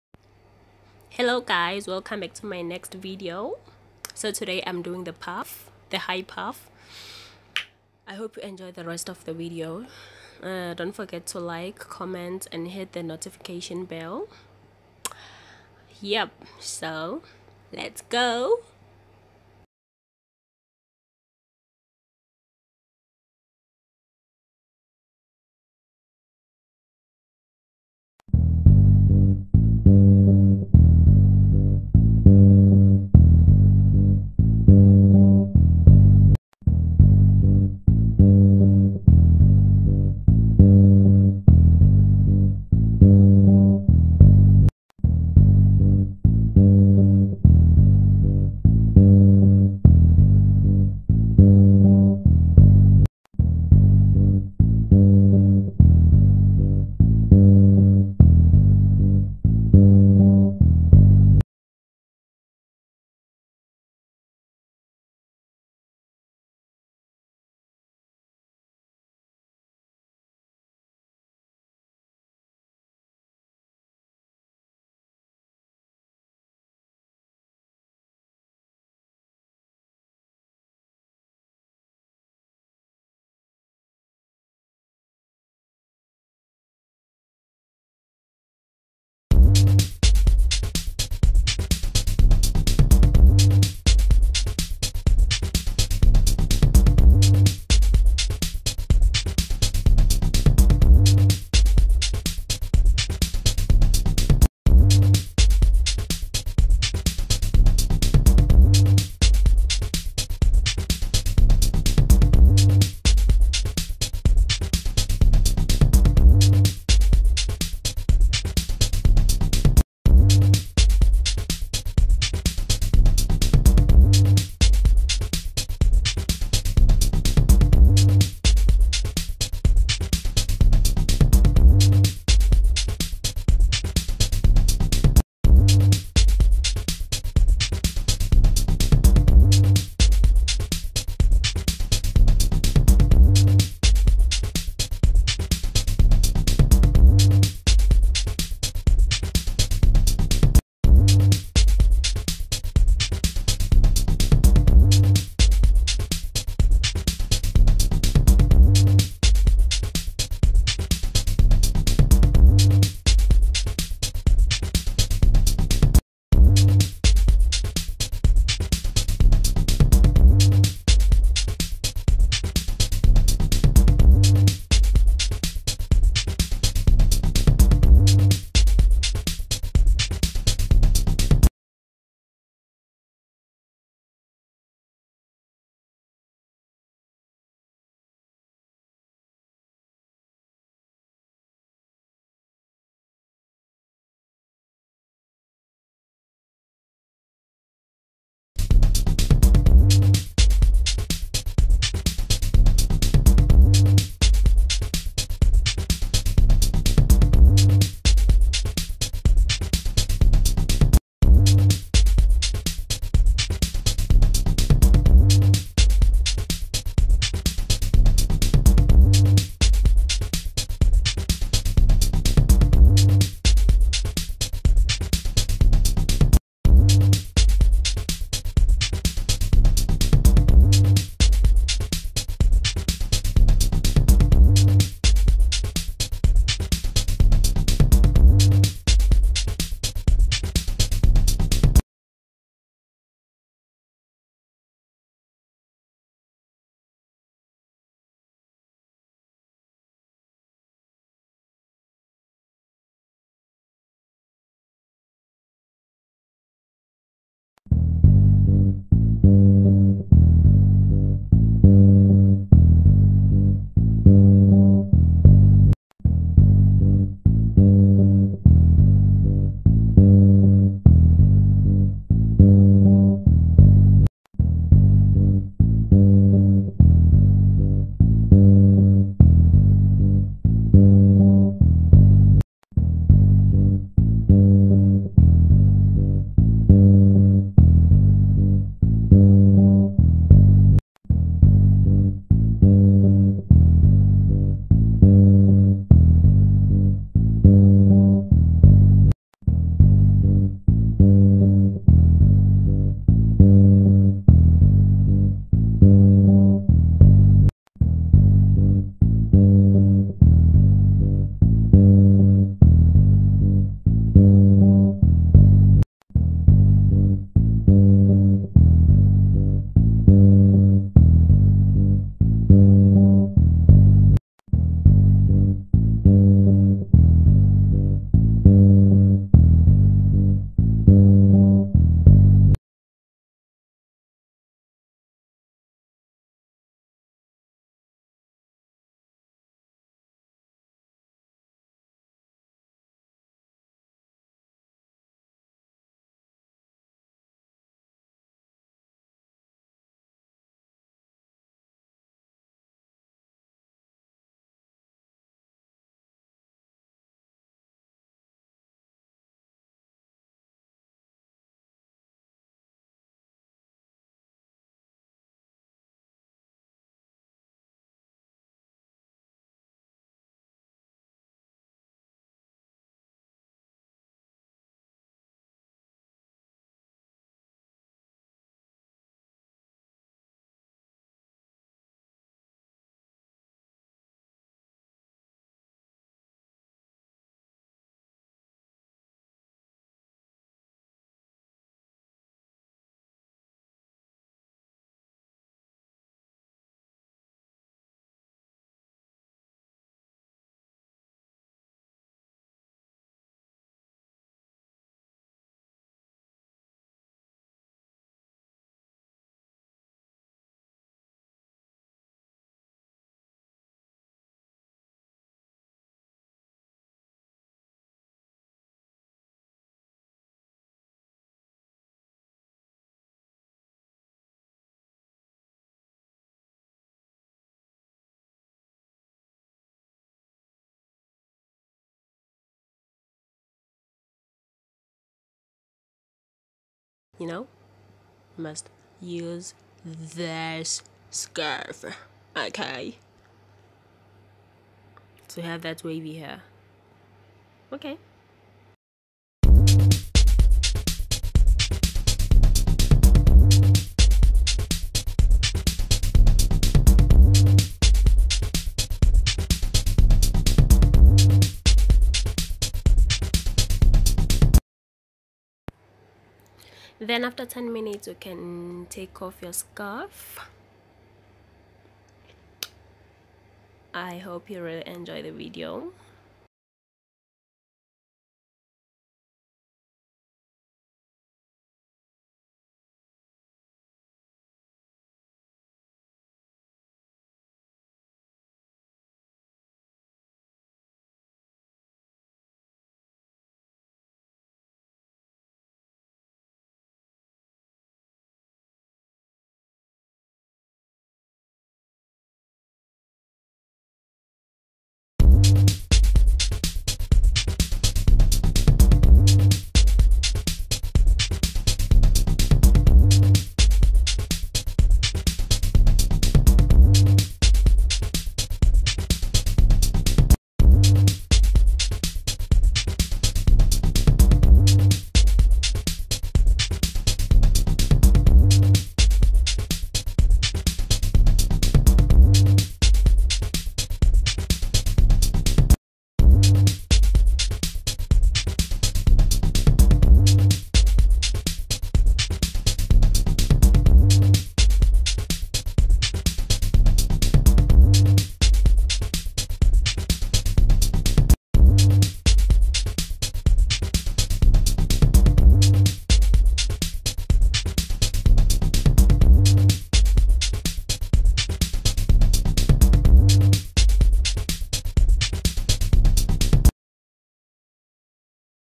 High Puff| Type 4 hair| Aunt Jackie's Hair Gel
Hey loves welcome back to another video, I've been in love with the Aunt Jackie's products. you should try it it's amazing!
Catch me on my other platforms